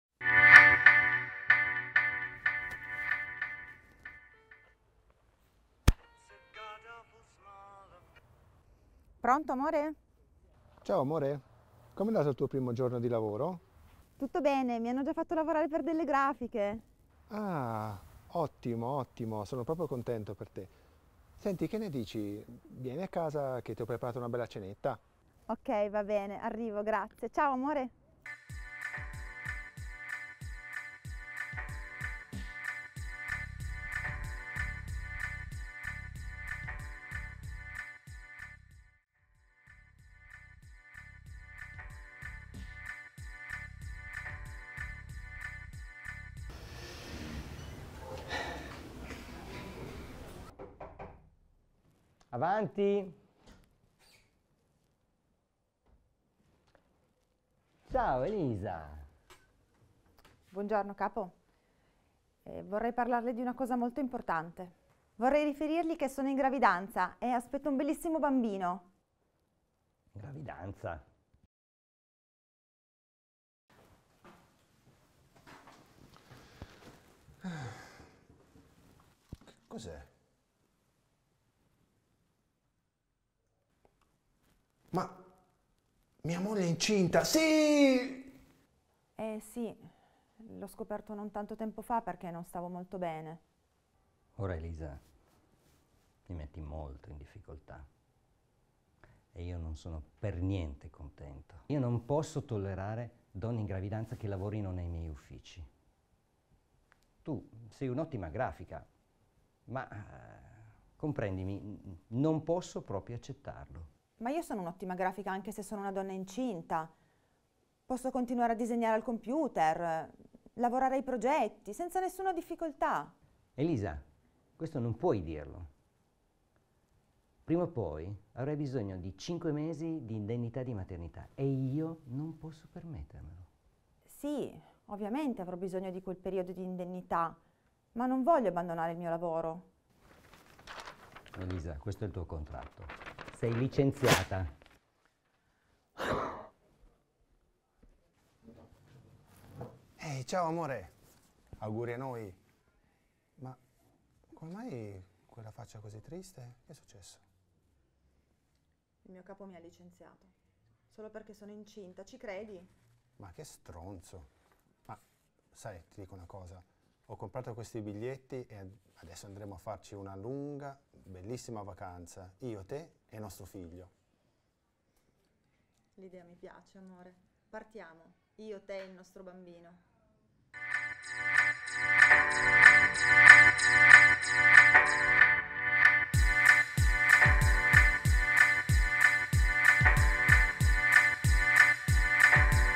Fai la tua parte
Una riflessione sulle politiche di genere e sui diritti dei lavoratori e delle lavoratrici attraverso il cortometraggio realizzato dagli allievi dell'istituto G.Natta di Rivoli (To) nell'ambito del progetto Ambasciatori del Consiglio regionale 2022/23.